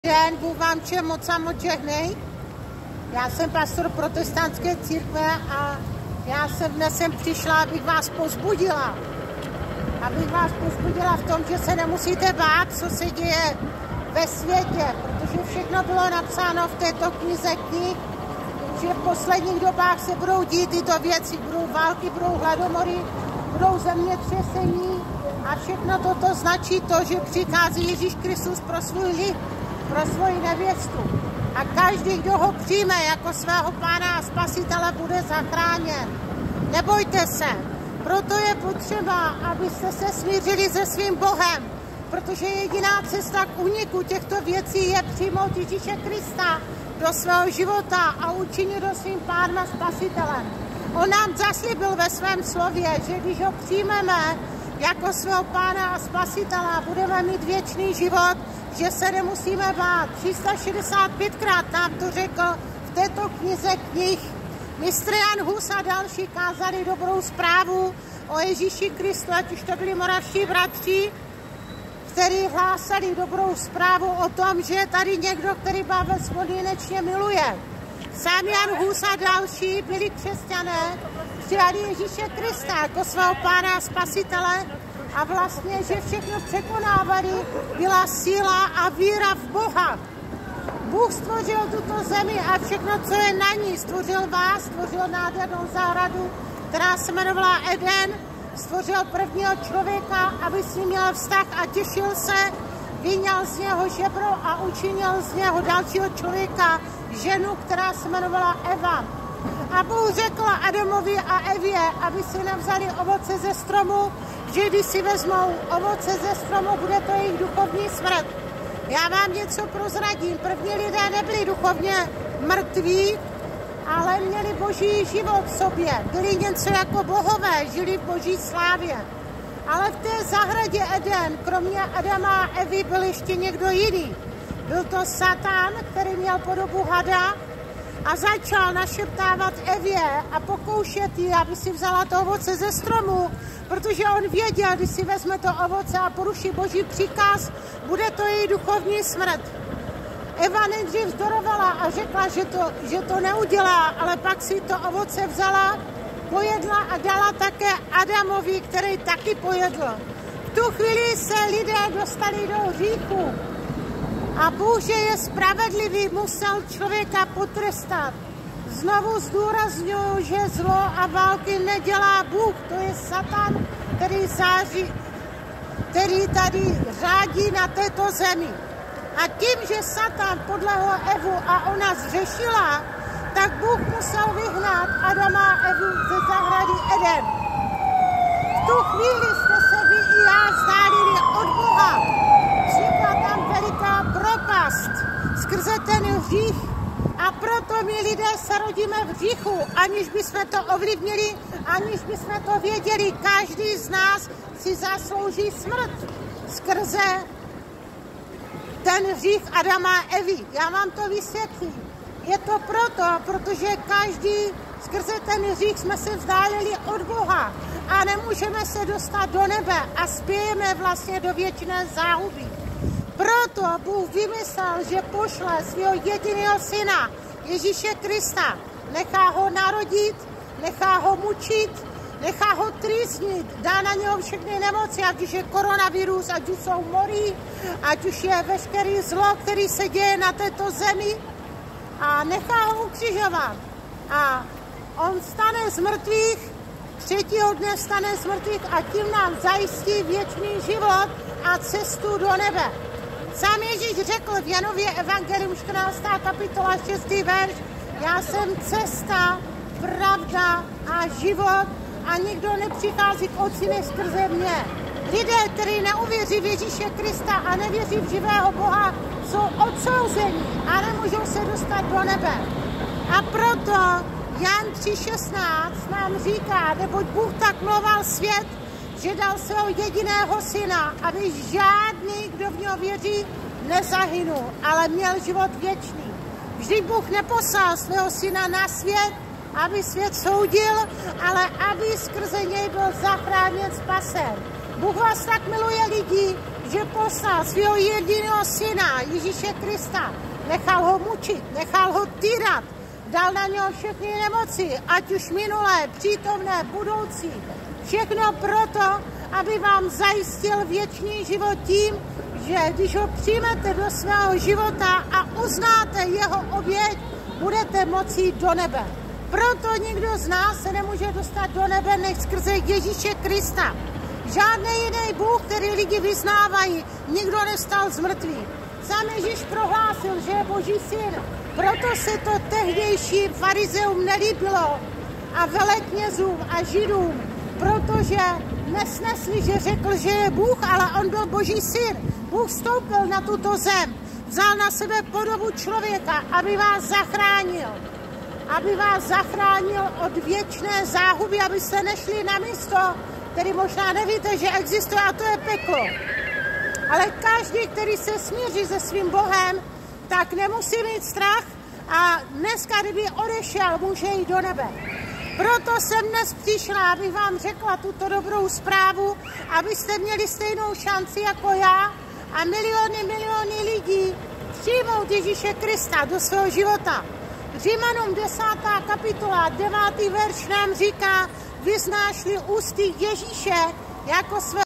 Slovo k současné situaci ve světě 2, Evangelizace 3.3.2022 náměstí Brno-Svatava Veverková
Křesťanské společenství Oheň probuzeni
Svatava Veverková/Služba/Ministry